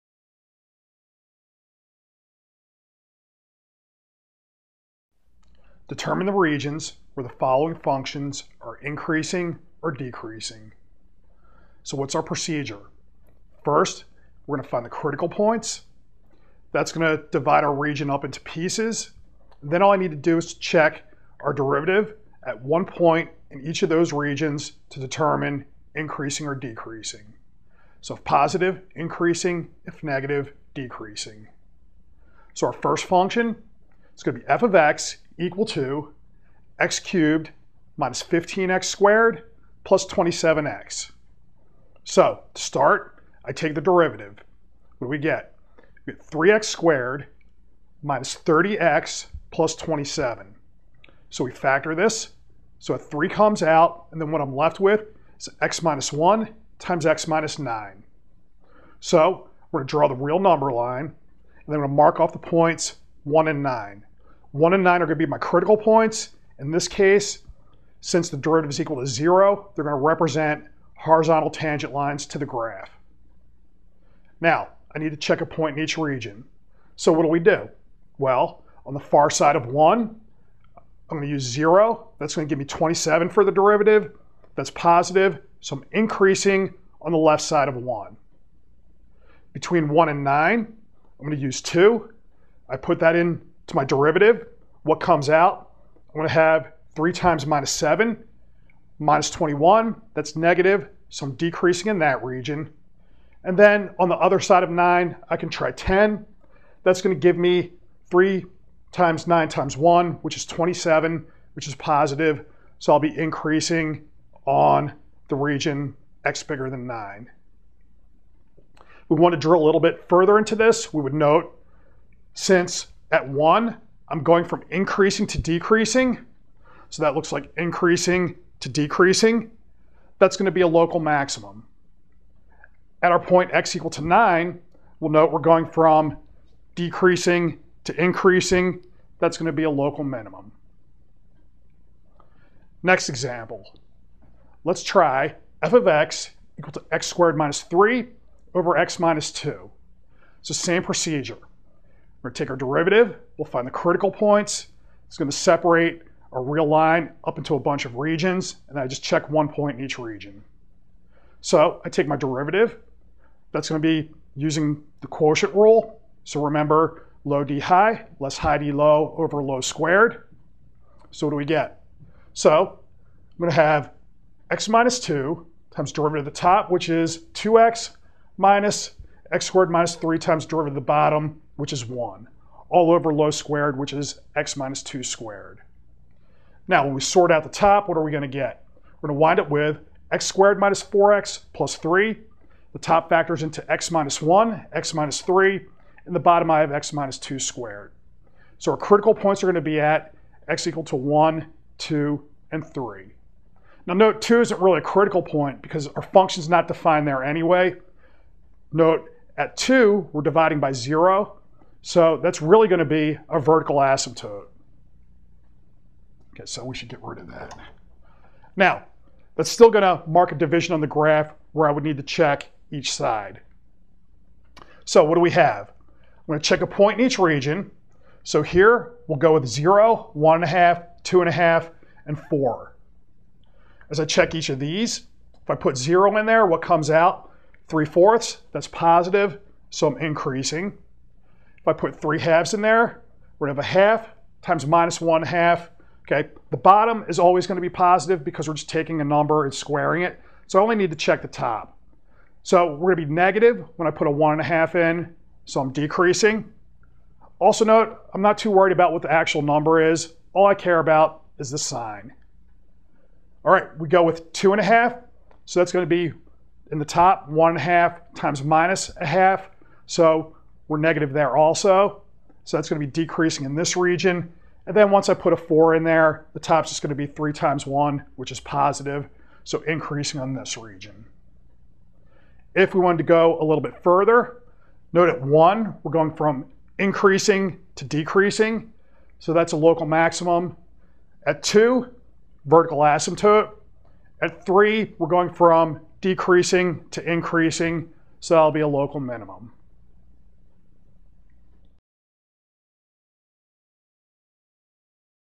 Example of Increasing/Decreasing 1
Calculus:  Find the regions of increasing and decreasing for the functions:  (a)  f(x) = x^3 - 15x^2 + 27x,   and   (b)  f(x) = (x^2 - 3)/(x-2).  Then determine whether the critical points are local minimums, local maximums, or neither.